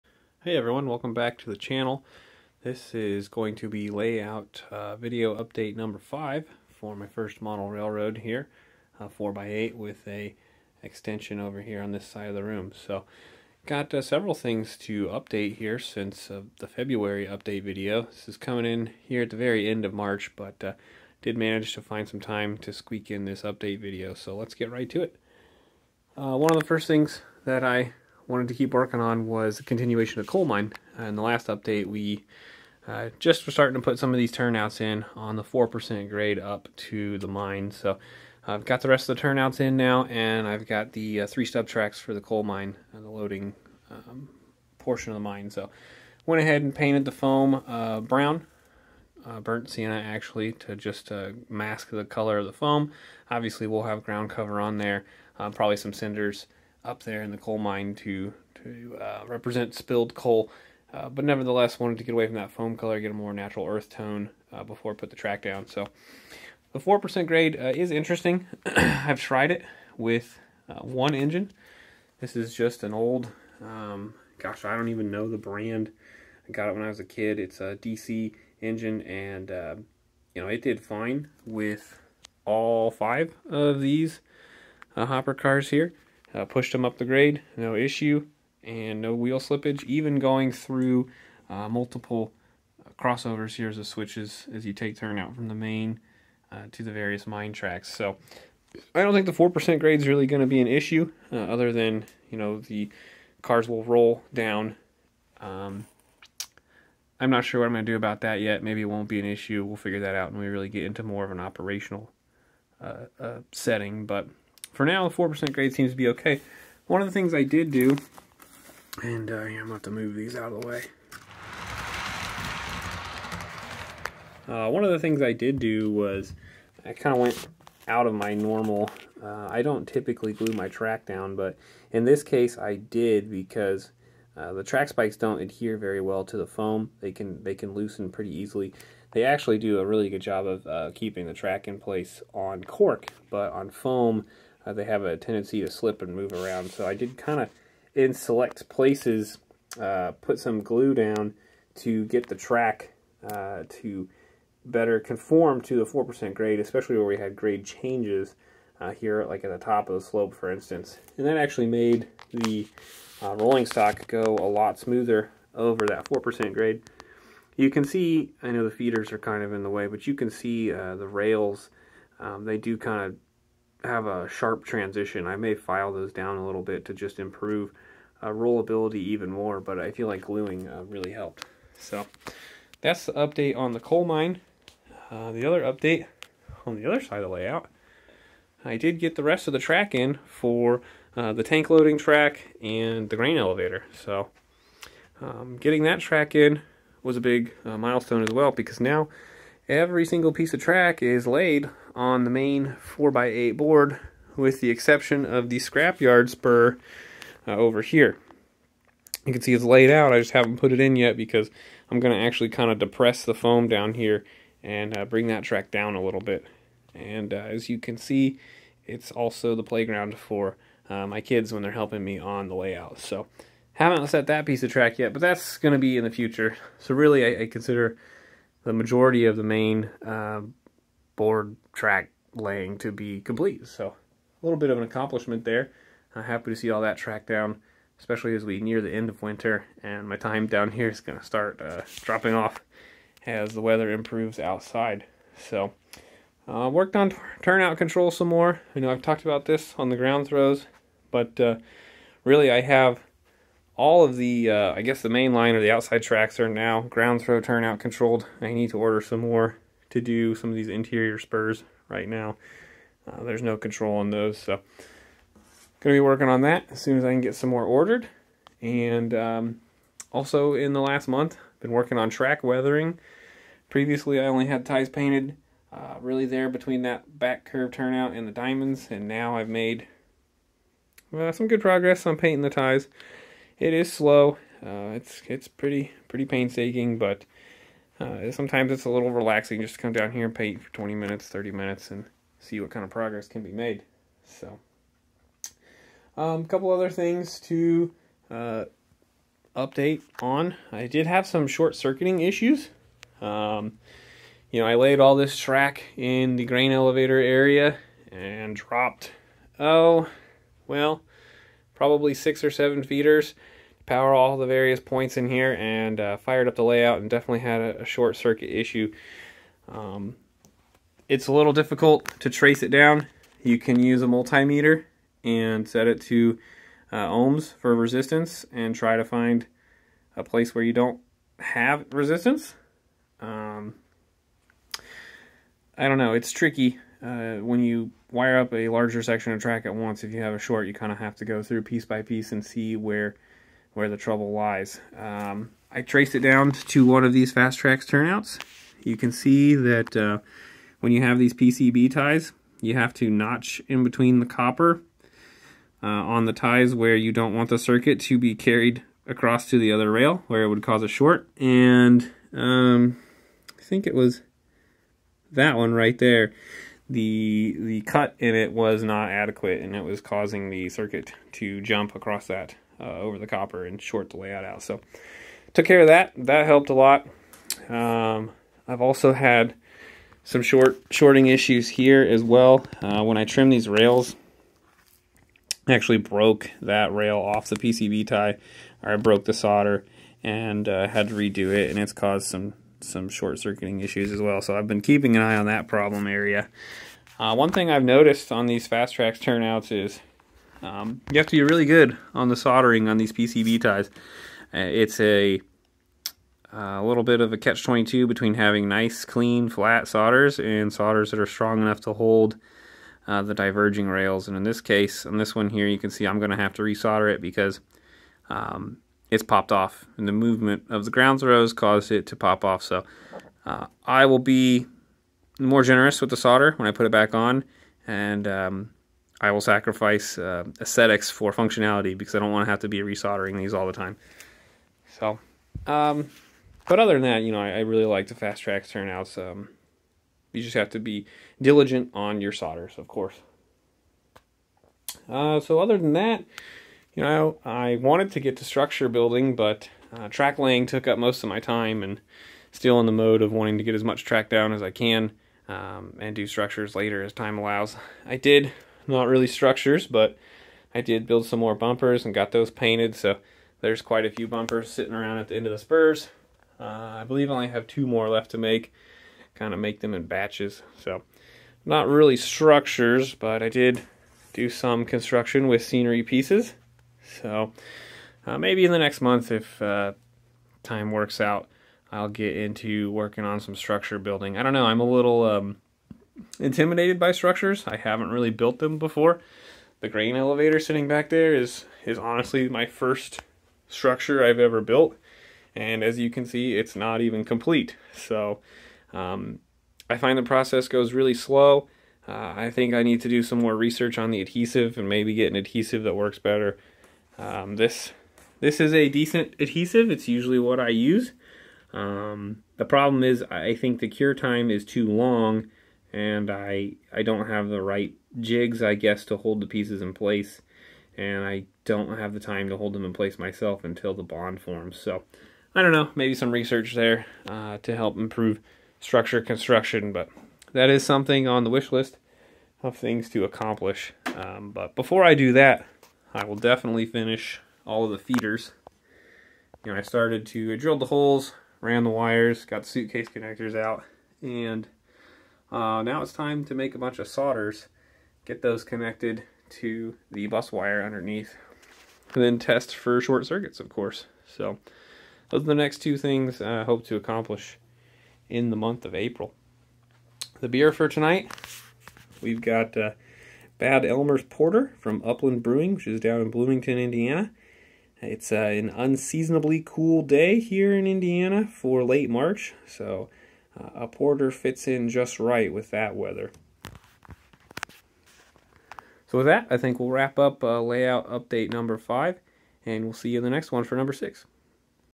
Layout Update 5 Mar. 2022 - More Track Laying, Turnout Repair, and Short Circuits!
Grain elevator and coal mine tracks are down, including the 4% grade up to the mine. We also deal with short circuiting and soldering repairs on turnouts.

It's been one year since Layout Update 2 and the model railroad looks completely different. From just the bench work in update 2 to 95% of all track on the main board laid in update 5.

The beer this month is a porter from right here in Indiana.